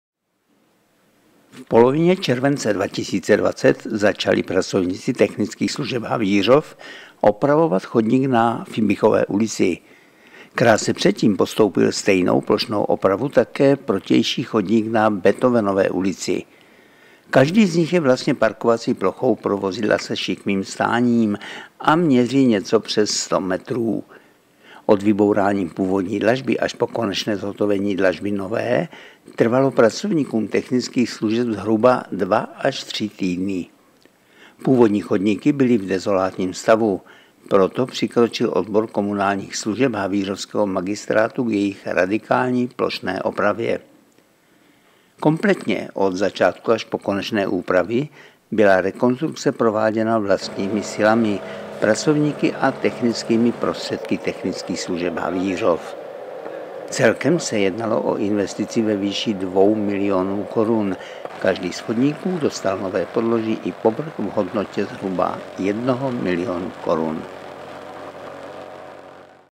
Oprava chodníku na ulici Fibichova - 17.- 31. července 2020
Některé chodníky ve městě vyžadují nutnou opravu a letní období je tím nejlepším k jejímu provedení.